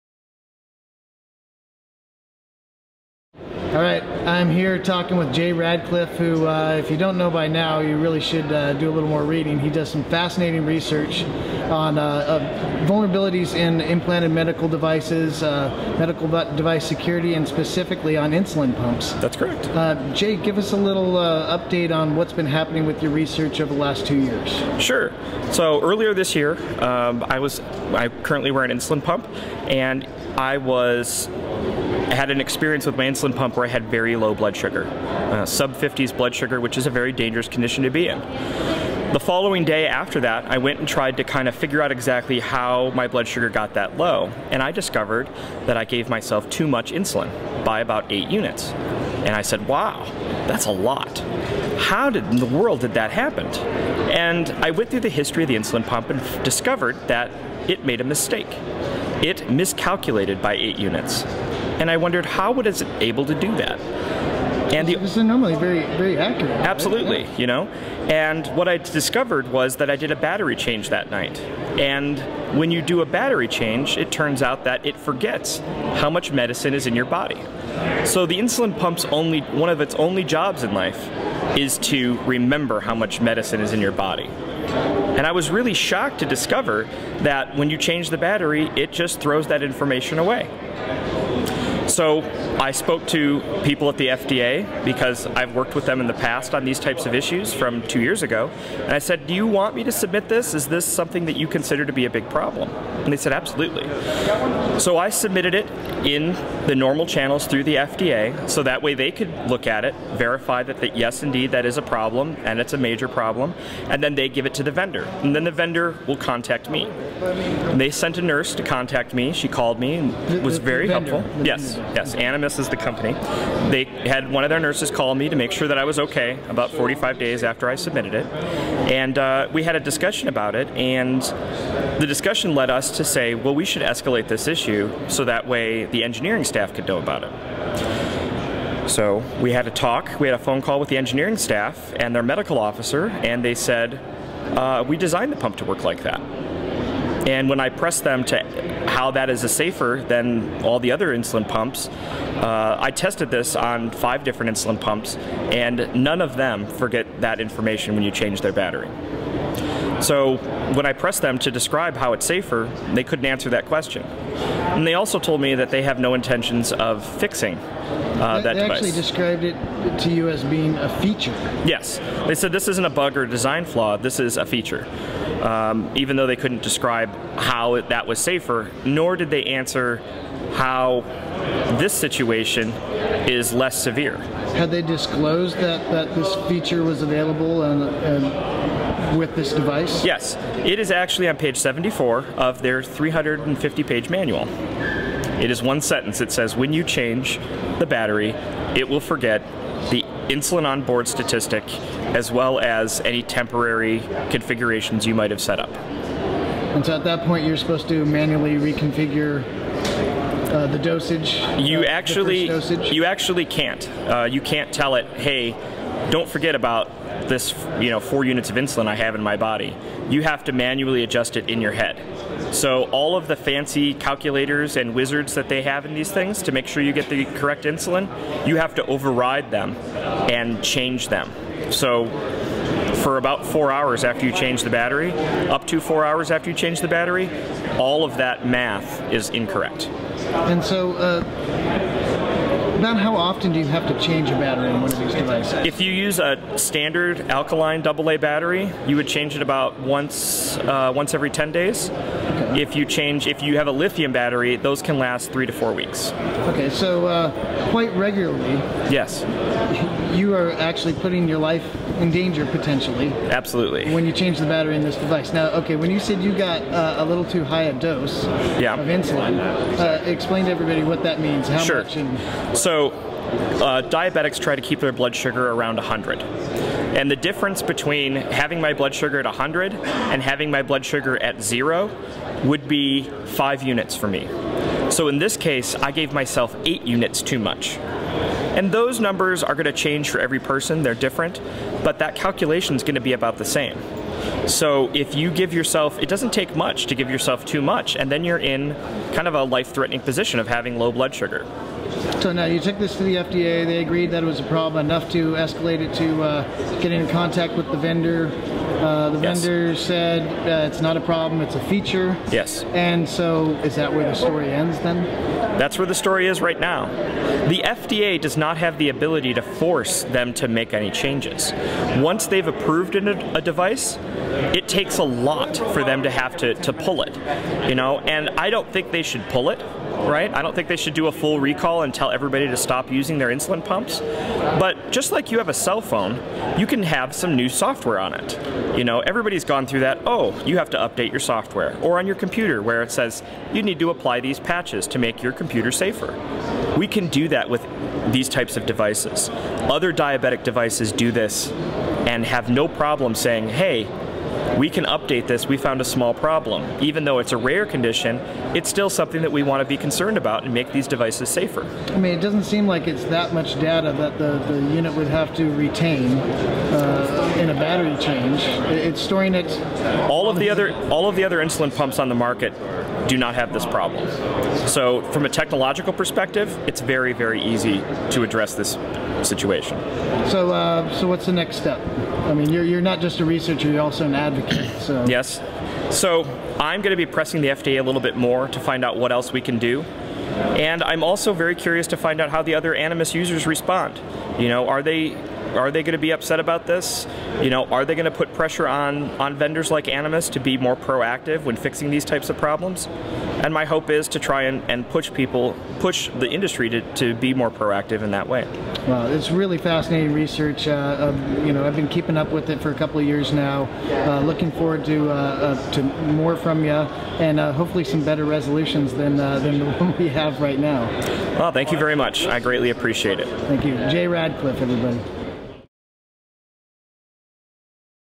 Jay Radcliffe on Implanted Medical Device Security
Jay Radcliffe (@jradcliffe02) has been in the security business for over twelve years, and is currently a Senior Security Analyst for InGuardians.

Radcliffe, a diabetic, made headlines two years ago when he gave a presentation on weaknesses in diabetic medical devices, how similar they were to industrial SCADA systems, and how they can be hacked.

This year at BSidesLV, Radcliffe gave another very intriguing talk where he performed a live demo of a critical software flaw that has never been discussed publicly.

Aside from his research into the buggy device, Radcliffe also discussed how there is no real imperative for the device manufacturers to remedy such flaws, even though they may put the users of these devices in harms way, including the possibility of death.

Radcliffe also discusses the limitations that agencies like the FDA have when addressing these kinds of vulnerabilities, and how progress has been made in the vetting of many medical devices where security is concerned...